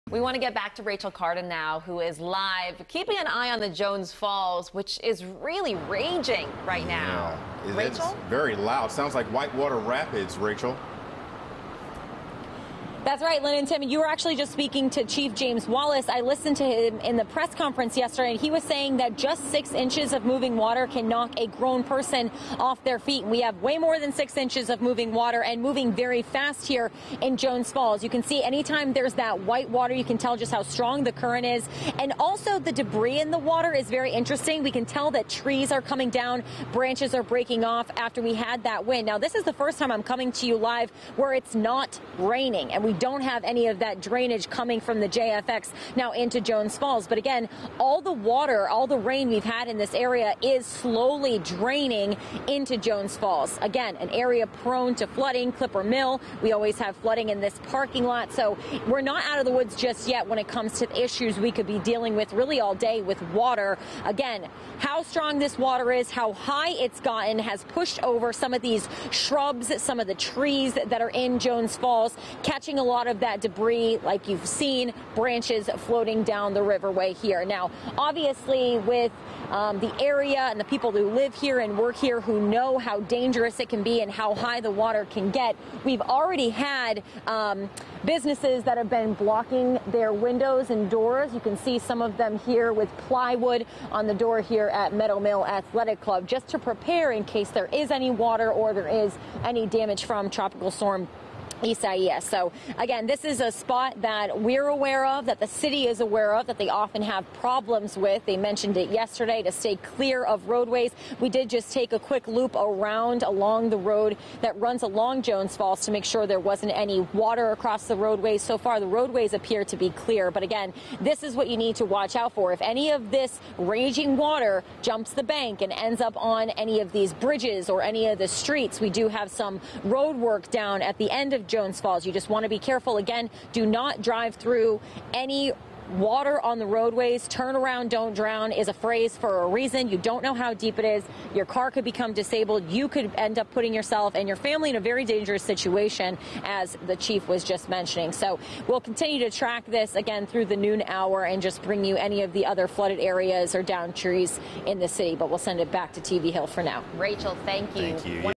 WJZ's Rachel Cardin Update On Jones Falls Following Tropical Storm Isaias
WJZ reporter Rachel Cardin is live in Jones Falls with the latest on how the area is faring with Tropical Storm Isaias moving through.